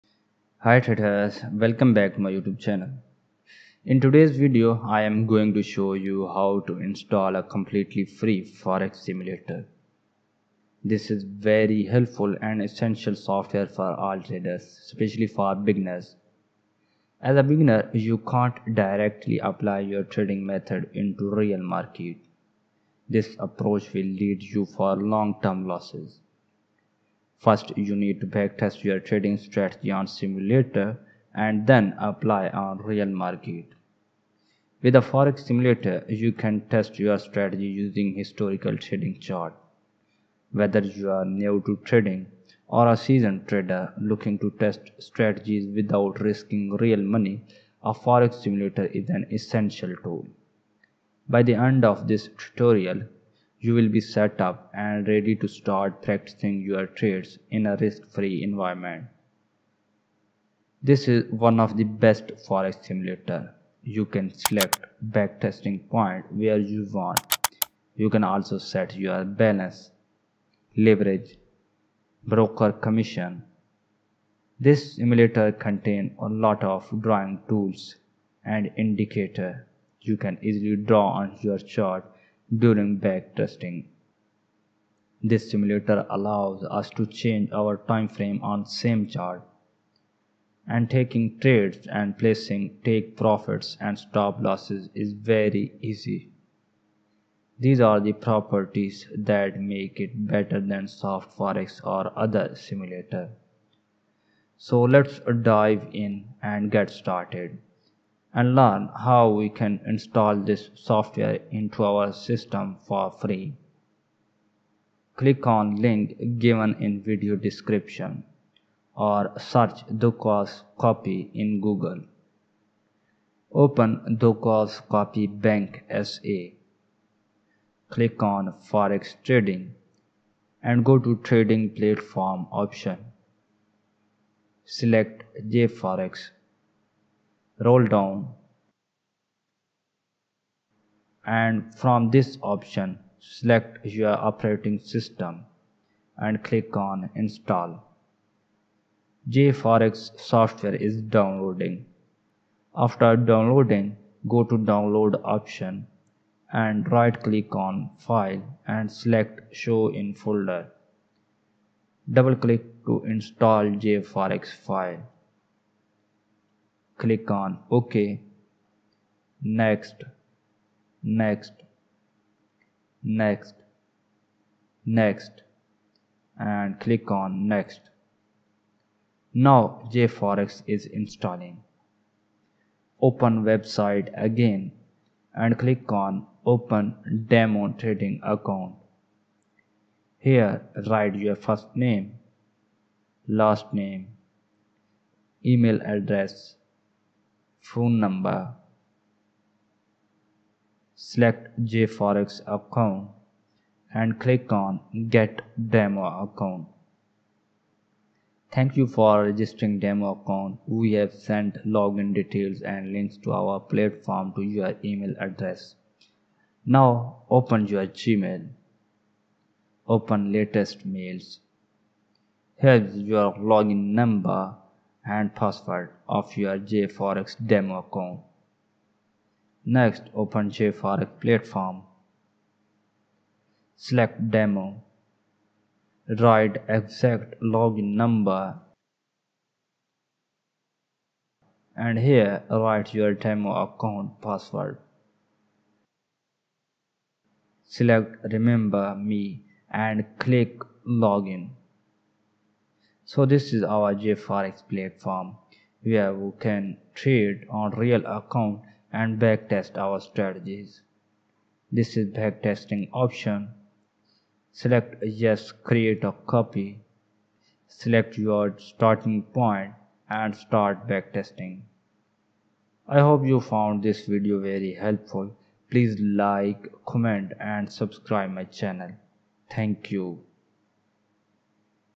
✅ Free Simulator for forex, Crypto & stocks| backtest your trading strategy with free simulator
Hi traders ! In this video i am going to show you how you can download and install free forex, stocks and crypto simulator and backrests you strategy.
In today's video, i am going to show you how to install a completely free forex simulator. this is very helpful and essential software for all traders specialty for beginners. AS a beginner you cant directly apply your trading method into real market this approach will lead you for long term losses. first you need to backtest your trading strategy on simulator and then apply on real market. with a forex simulator your can test your strategy using historical trading chart. Whether you're new to trading or a seasoned trader looking to test strategies without risking real money, a forex simulator is an essential tool. By the end of this tutorial, you'll be set up and ready to start practicing your trades in a risk-free environment. . This is one of the best forex simulator. You can select back testing point where you want. You can also set your balance leverage broker commission. this simulator contains lot drawing tools and indicator you can easily draw on your chart during back testing. this simulator allows us to change our timeframe on same chart. and taking trades and placing take profit and stop loss is very easy. these are the properties that makes it better then sof4x or other simulator. so Let's dive in and get started! and learn how can install this softwaare into our system for free. click on link given in video description or search Dukascopy in google. open "dukascopy bank SA". Click on Forex trading and go to trading platform option. select Jforex. roll down and from this option select you operating system. and click on install. jforex software is downloading. after downloading go to download option and right click on file and select show in folder. double click to install jforex file. OK, Next, Next, Next , Next , Next . Now jforex is installing. Open website again and click on open demo trading account. Here write your first name , last name , email adress , phone number. select jforex account and click on Get demo account. thank you for registering demo account we have sent login details and links to our platforms to your email adress. Now open your gmail. open latest mail. here is your login number and password of your jforex demo account. Next Open jforex , select demo. write exact login number and here write your demo account password. select remember me and click log-in. So this is our jforex platform were can trade on real account and backtest our strategies. this is backtesting option. select yes creat a copy. select your starting point and start backtesting.. i hope you found this video very helpful. please like comment and subscribe my channel. thank you